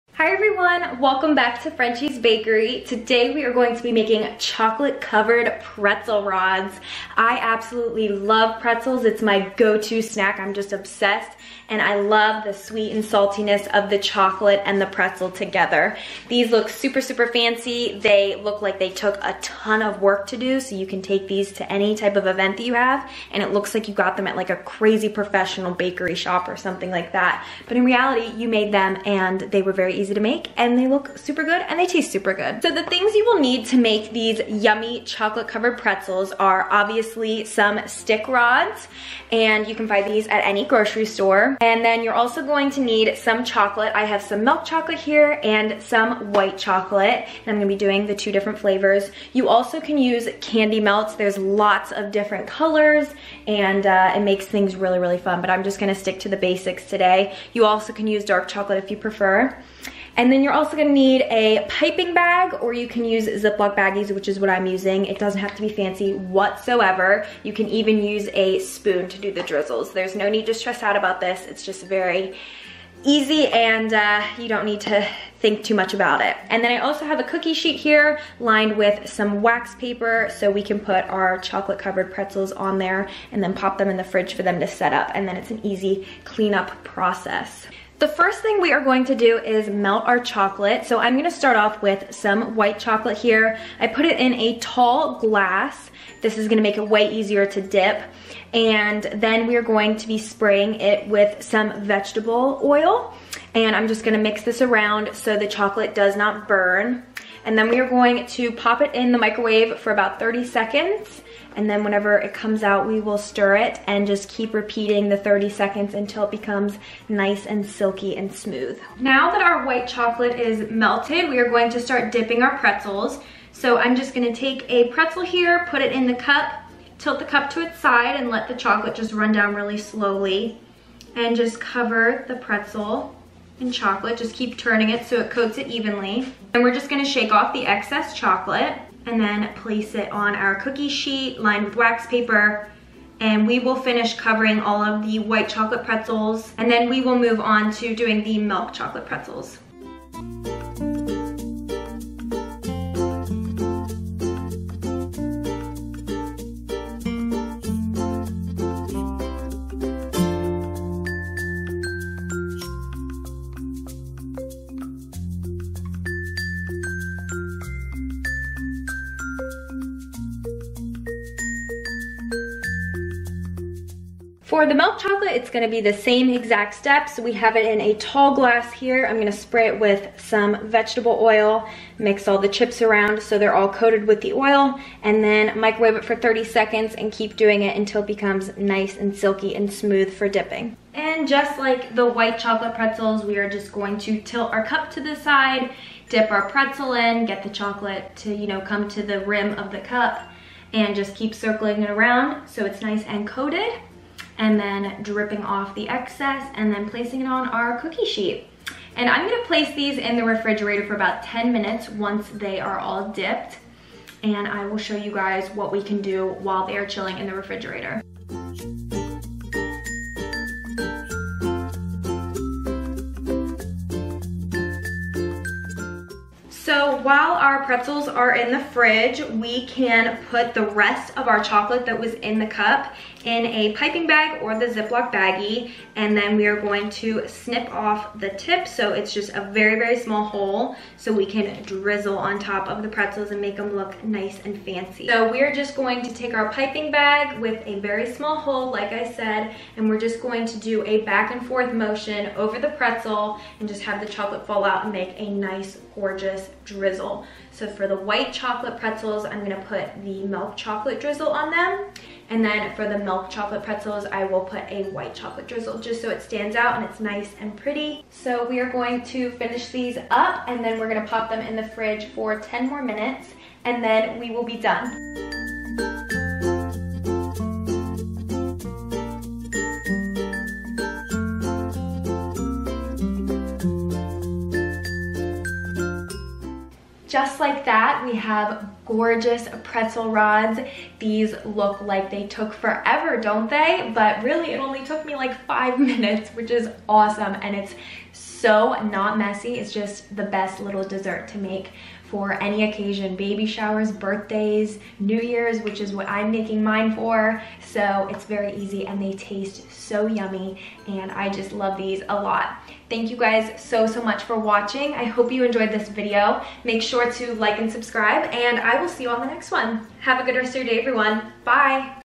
CHOCOLATE COVERED PRETZEL RODS
Learn how to make chocolate covered pretzel rods. Super easy and yummy!

Hope you all enjoyed this video! Lots of more content and fun coming soon.